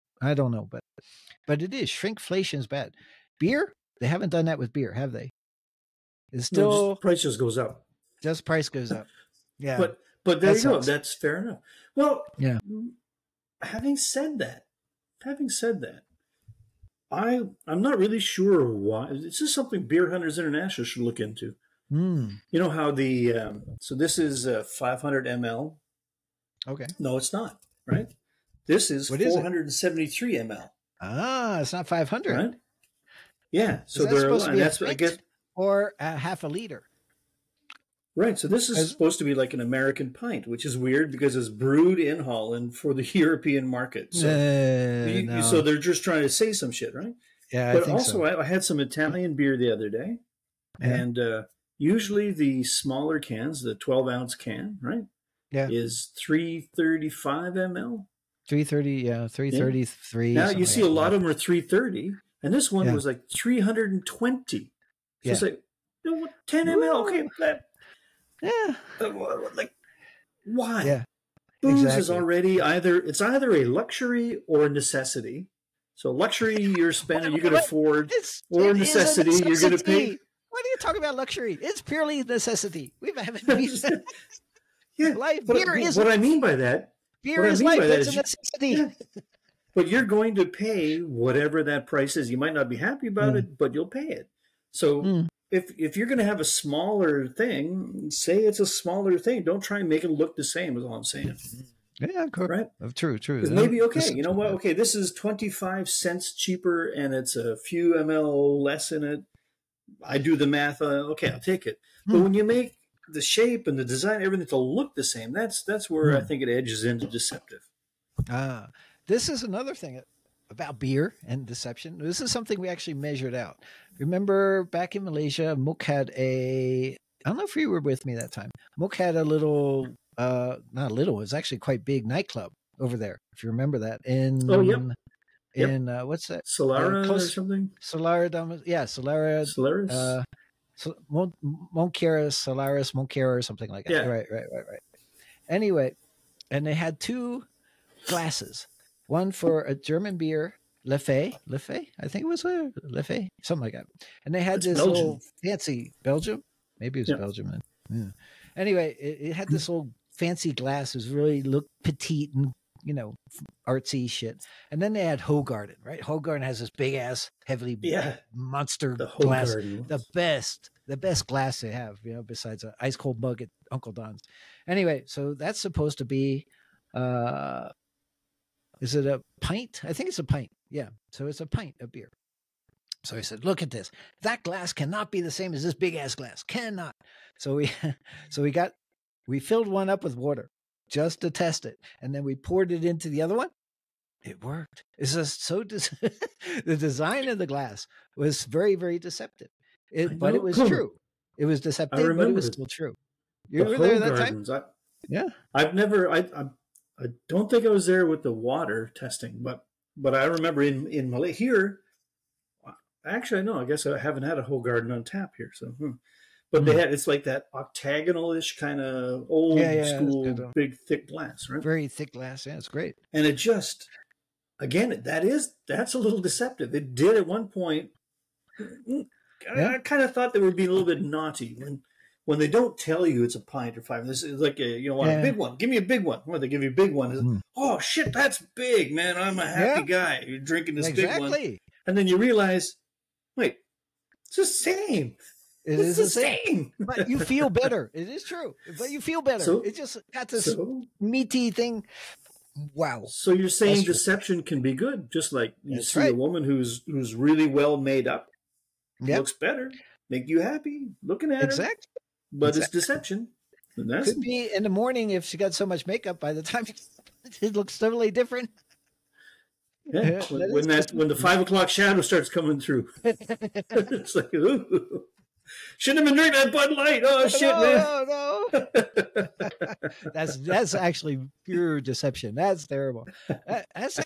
Deceptive Women or Beer ? (Wise Old Bastards)
Deception:  Beautiful women or a beautiful ice cold beer ?
Beer never argues & aways there to please !
How about a beautiful woman serving beer ?
Beer is Life  &  Life is  Good !!!